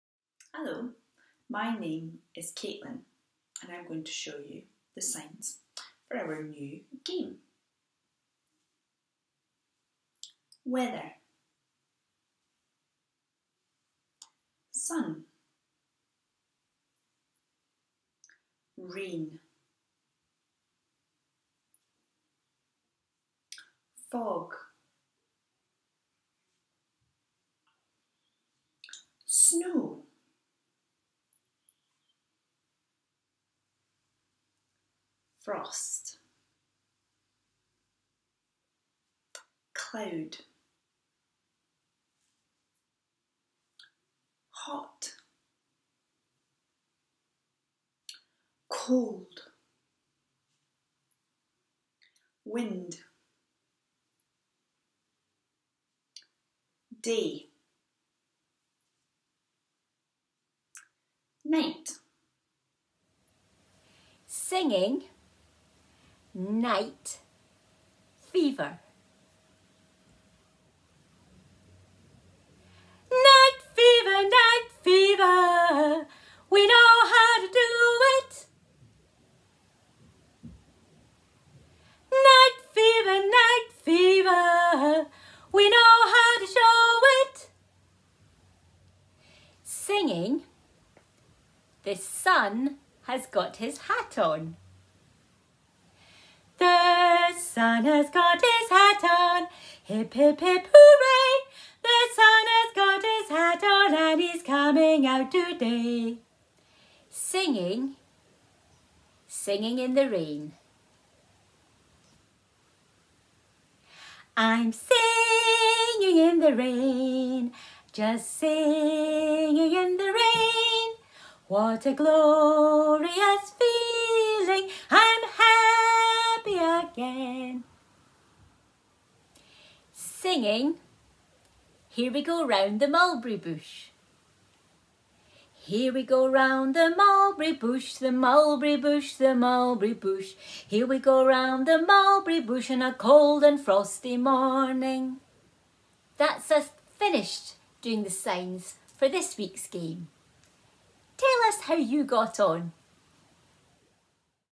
Makaton signs and songs for the 'Weather Bingo' game
Here are the signs for the 'Weather Bingo' game that we have emailed out this week! Playing this game will help to support communication skills such as naming, turn taking and of course signing!

You might be watching because you are interested in learning some new vocabulary in Makaton. This video contains signs for weather/environment including: sun, frost, wind, hot and cold.

Here are our colleagues showing you lots of signs you can use while playing the game. We've added a wee (optional) twist - some bingo calling cards ask you to sing a song that includes the weather sign! Let us know how you got on in the comments - we'd love to hear from you!

 Remember to always *speak and sign all the time!*